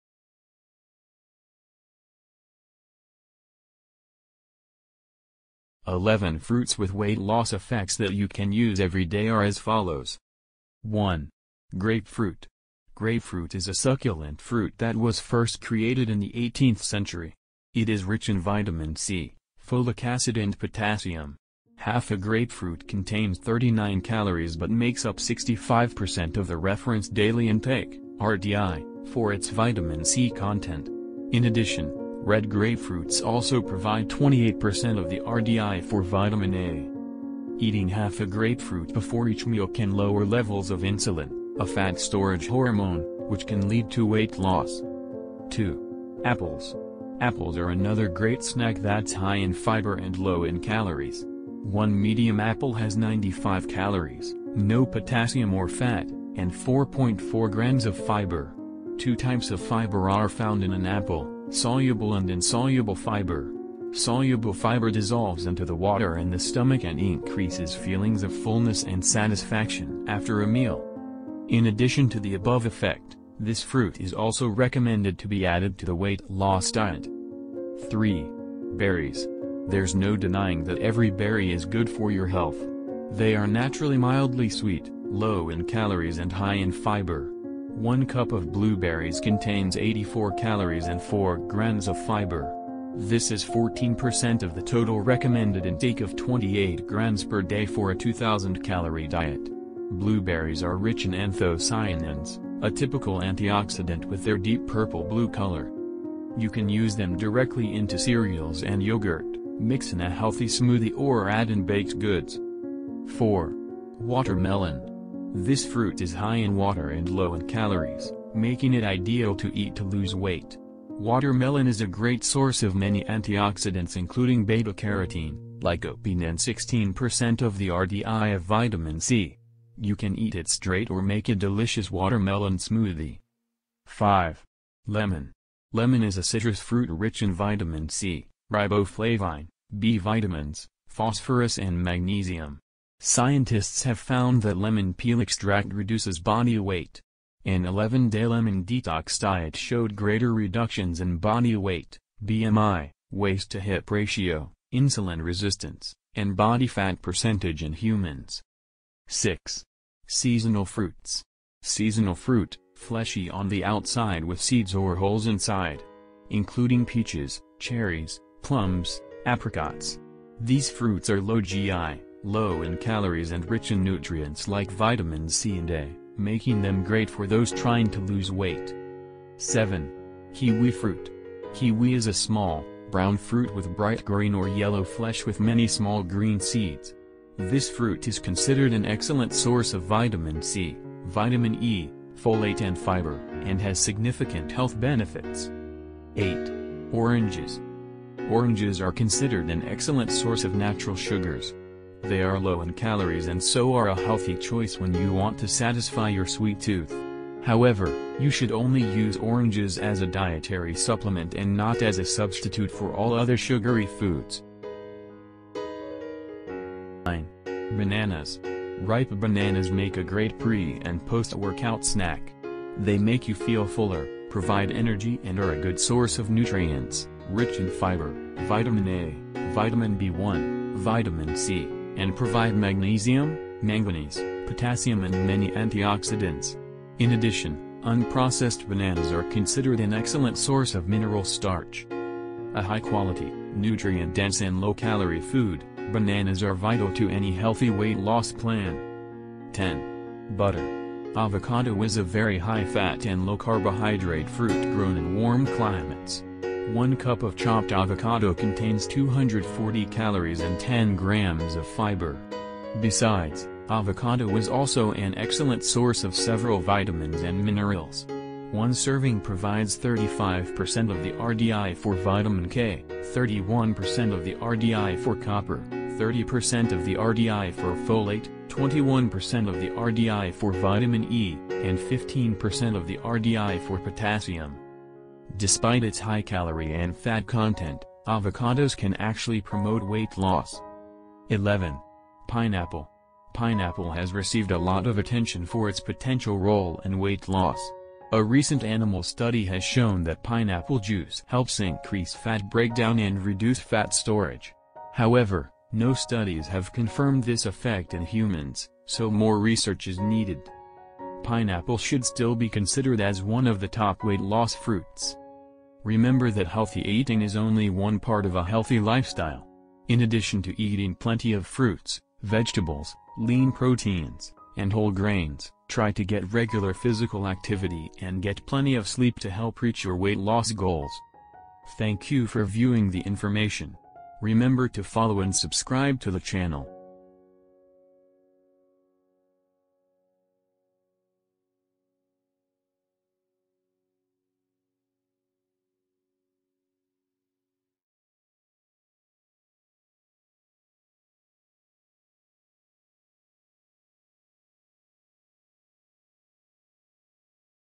Weight Loss Fruit - 11 Best Fruits for Weight Loss
11 fruits with weight loss effects that you can use every day are as follows:

1. Grapefruit 0:10
2. Apples 0:45
3. Berries 1:17
4. Watermelon 1:52
5. Lemon 2:13
6. Seasonal Fruits 2:38
7. Kiwi fruit 2:59
8. Oranges 3:15
9. Bananas 3:36
10. Avocado 4:09
11. Pineapple 4:50

Remember that healthy eating is only one part of a healthy lifestyle. In addition to eating plenty of fruits, vegetables, lean proteins, and whole grains, try to get regular physical activity and get plenty of sleep to help reach your weight loss goals.

Thank you for viewing the information.
Thank you - Wish you always healthy.
☘️ FOLLOW DAGZOG
© Copyright by Dagzog - Do not Reup.